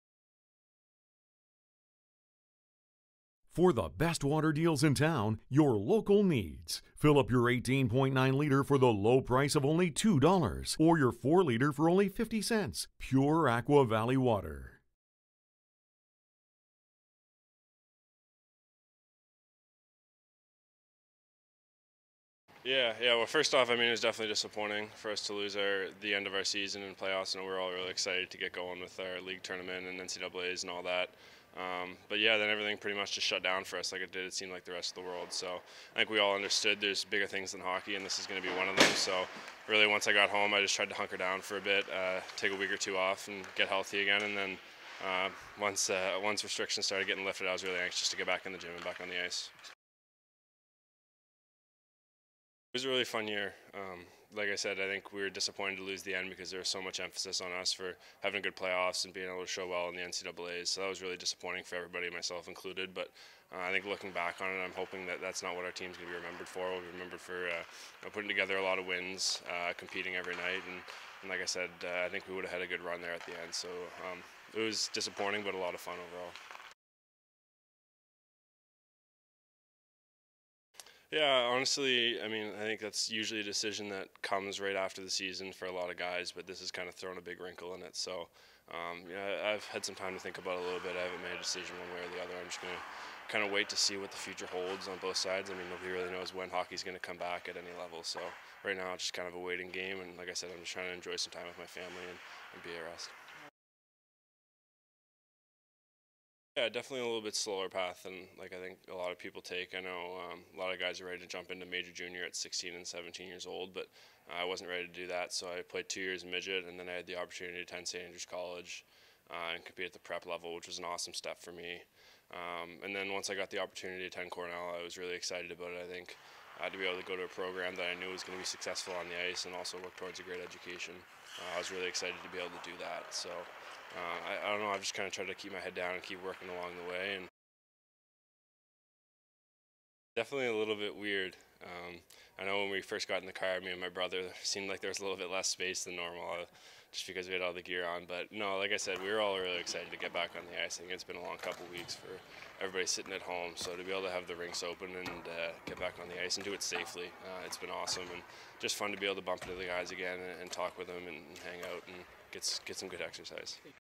Morgan Barron nears a decision
New York Rangers prospect Morgan Barron is coming off an exceptional junior season with Cornell Big Red. The 21 year old was an 1st team  All American and helped Big Red be consistently ranked among the top 10 teams in the NCAA.  Barron was one of top scorers and was named the ECAC Player of the Year and a finalist for the prestigous Hobey Baker Award.   He is back home in Nova Scotia and uncertain what his next step will be due to COVID-19.